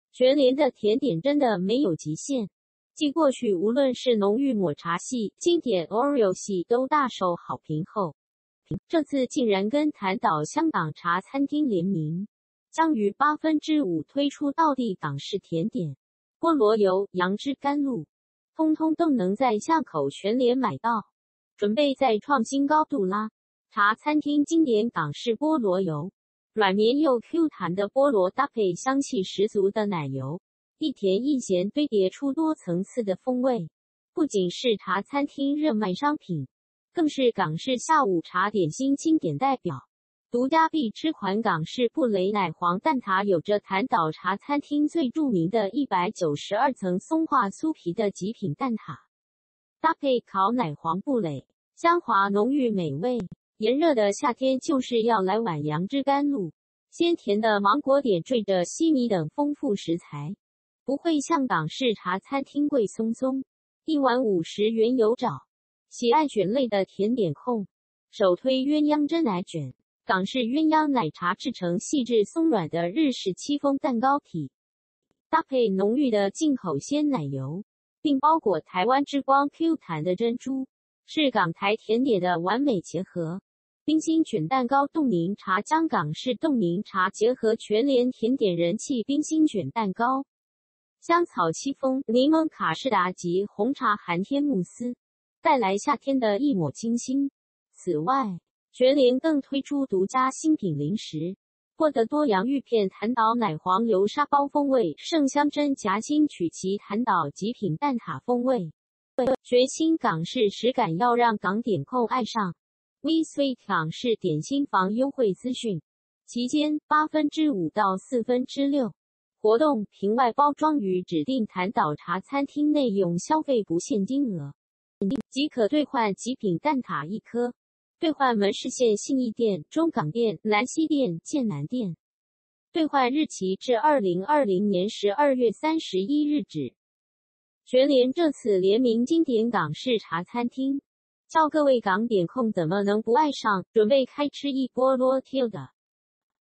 【全台】全聯WeSweet推道地港式甜點「楊枝甘露、菠蘿油」茶餐廳控必吃！
全聯的甜點真的沒有極限！繼過去無論是濃郁抹茶系、經典OREO系都大受好評後，這次竟然跟檀島香港茶餐廳聯名，將於5/8推出道地港式甜點，菠蘿油、楊枝甘露，通通都能在巷口全聯買到，準備再創新高度啦！
茶餐廳經典「港式菠蘿油」，軟綿又Q彈的菠蘿搭配香氣十足的奶油，一甜一鹹堆疊出多層次的風味，不僅是茶餐廳熱賣商品，更是港式下午茶點心經典代表。
獨家必吃款「港式布蕾奶皇蛋撻」有著檀島茶餐廳最著名的192層鬆化酥皮的極品蛋撻，搭配烤奶皇布蕾，香滑濃郁美味。
炎熱的夏天就是要來碗「楊枝甘露」，鮮甜的芒果點綴著西米等豐富食材，不會像港式茶餐廳「貴鬆鬆」，一碗50元有找！
喜愛捲類的甜點控，首推「鴛鴦珍奶捲」，港式鴛鴦奶茶製成細緻鬆軟的日式戚風蛋糕體，搭配濃郁的進口鮮奶油，並包裹台灣之光Q彈的珍珠，是港台甜點的完美結合。
「冰心捲蛋糕凍檸茶」 將港式凍檸茶結合全聯甜點人氣冰心捲蛋糕，香草戚風、檸檬卡士達及紅茶寒天慕斯，帶來夏天的一抹清新！
此外，全聯更推出獨家新品零食，「波的多洋芋片-檀島奶皇流沙包風味」、「盛香珍夾心曲奇-檀島極品蛋撻風味」，全新港式食感要讓港點控愛上！
【We Sweet 港式點心坊】優惠資訊
期間：5/8-6/4
活動：憑外包裝於指定檀島茶餐廳內用消費不限金額，即可兌換極品蛋撻一顆。（兌換門市限信義店/中港店/南西店/劍南店，兌換日期至2020/12/31止）
全聯這次聯名經典港式茶餐廳，叫各位港點控怎麼能不愛上？準備開吃一波囉～